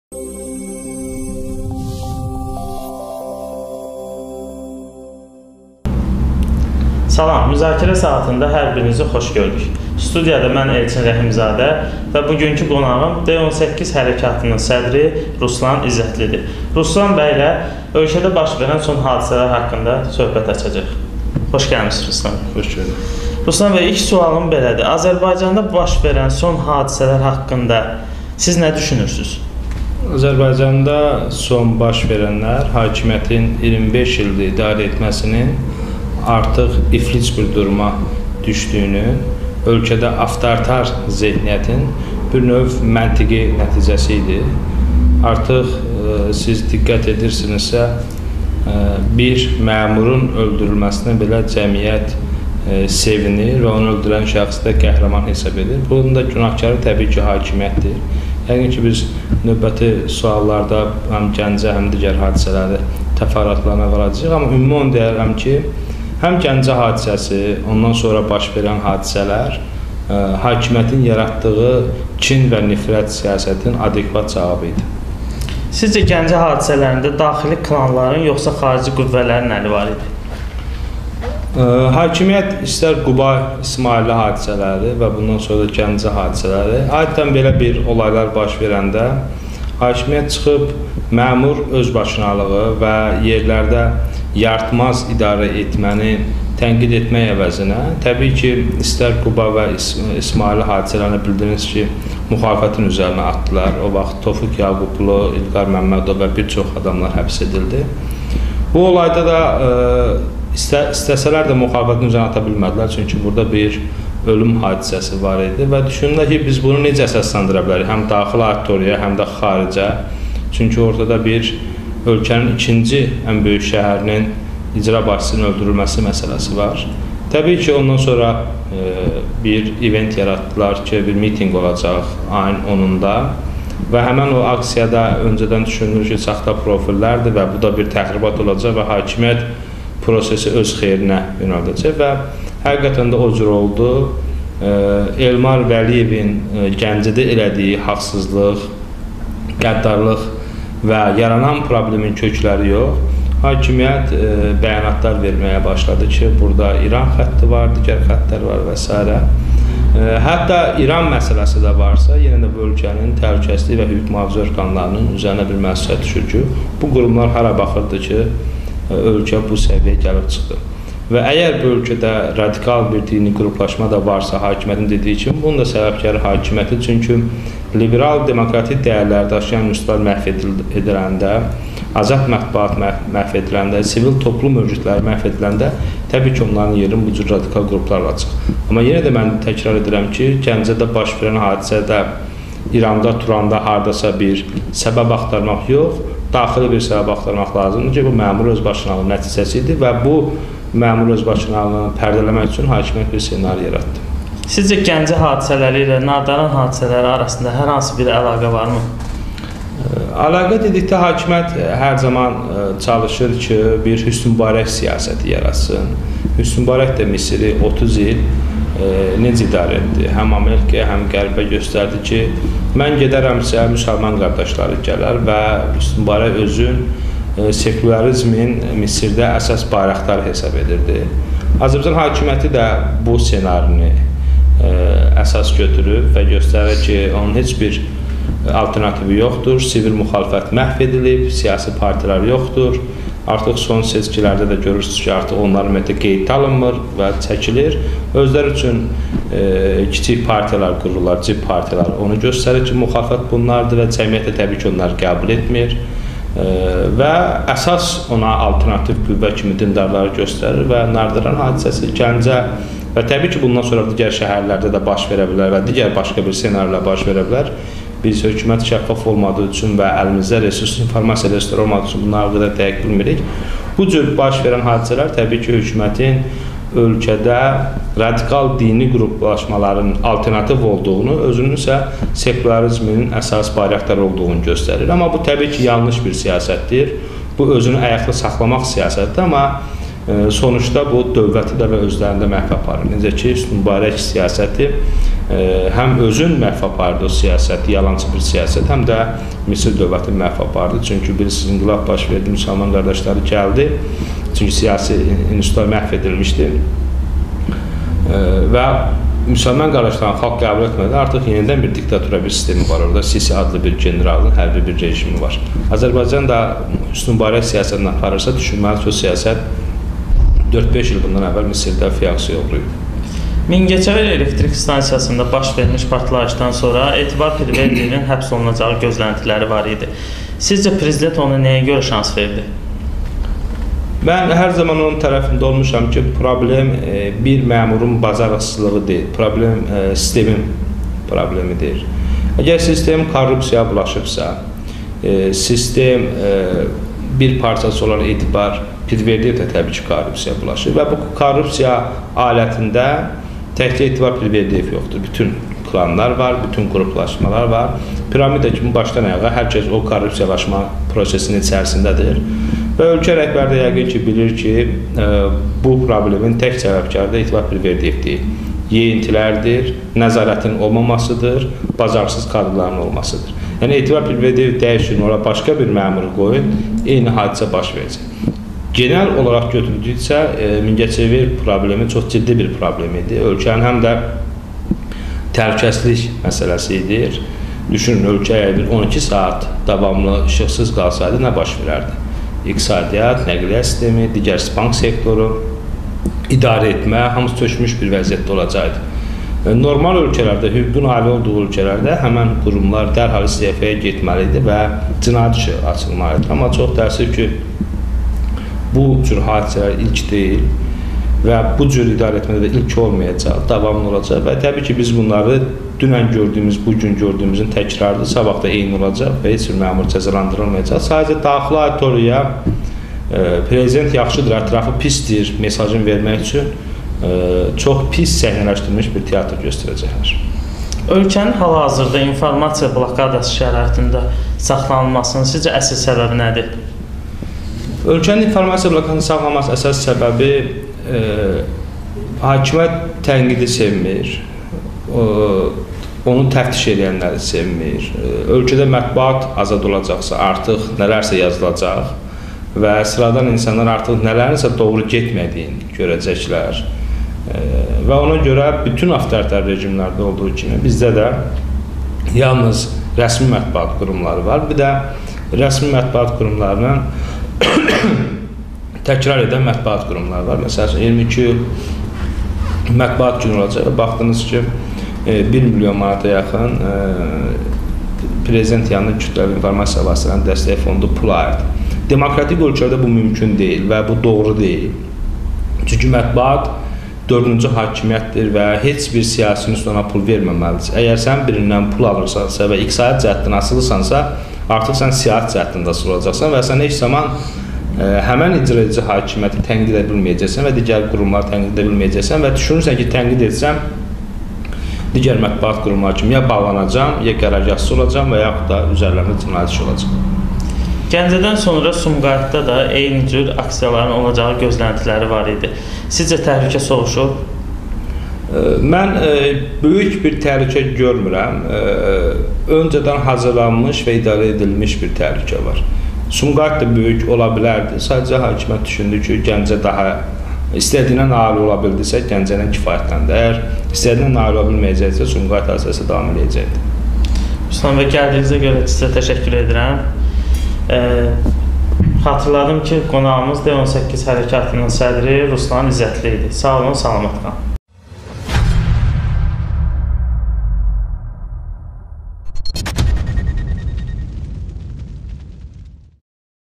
"Hakimiyyət Elmar Vəliyevin etdiyi haqsızlıqları İran xətti ilə ört-basdır etdi"-Ruslan İzzətli
"Müzakirə Saatı"-da "D18 Hərəkatı"-nın sədri Ruslan İzzətli ilə ölkədəki vəziyyət müzakirə olunur.
Ruslan İzzətli:" Bütün baş verən hadisələr hakimiyyətin 25 illik avtoritar rejiminin nəticəsi idi. Təhlükəsizlik qurumları hara baxırdı ki, ölkədə bu cür hallar baş verir? Gəncə hadisəsi məmur özbaşınalığının nəticəsi idi".